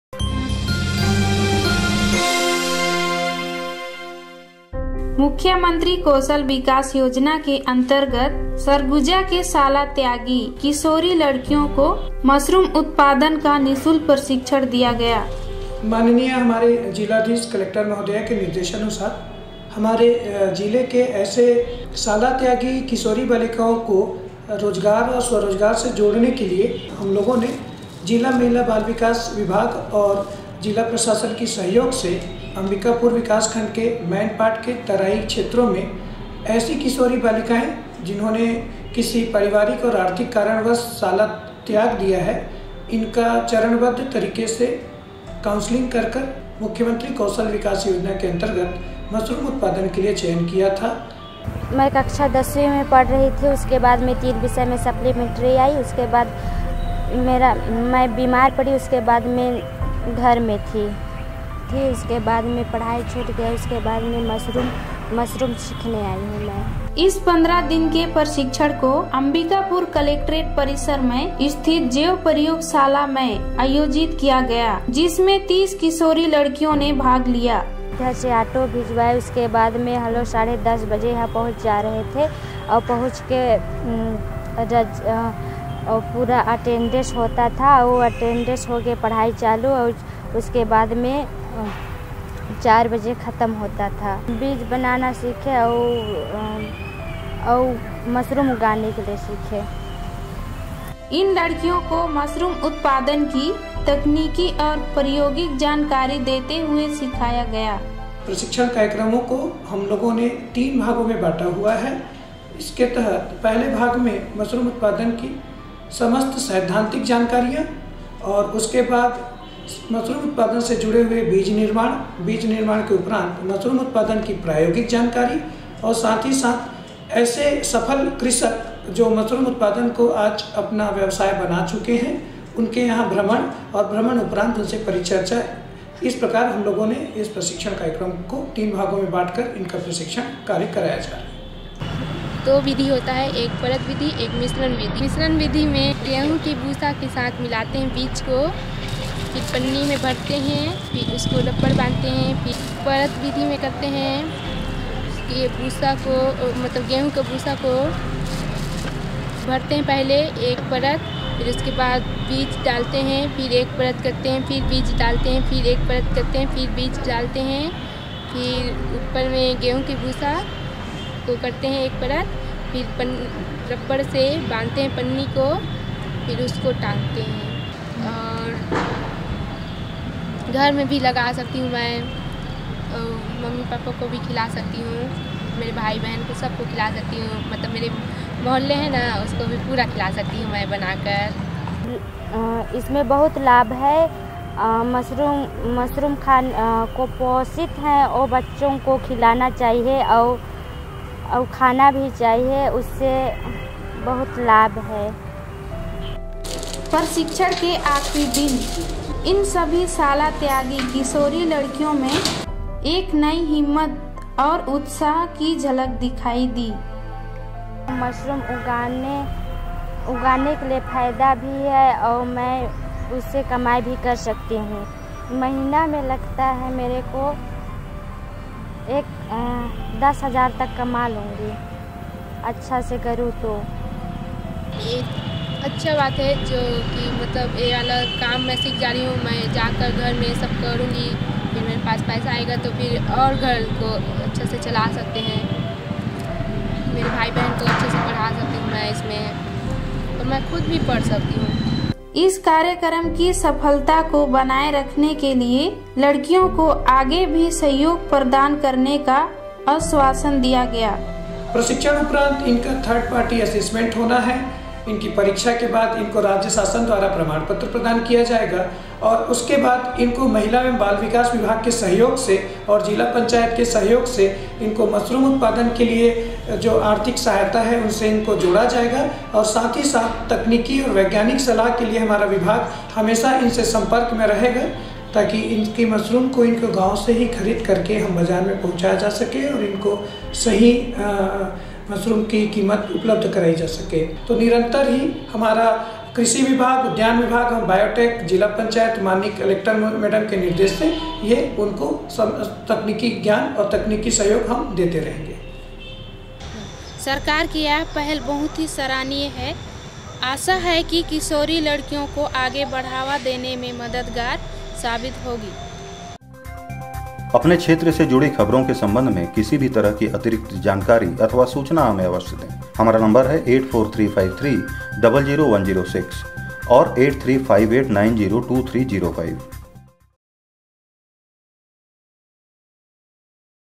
Mushroom Cultivation Training (Kaushal Vikas Yojana)
Adolescent school dropout girls of Surguja received training under the Chief Minister's Skill Development scheme. Our correspondent Nankuwar brings you the ground report.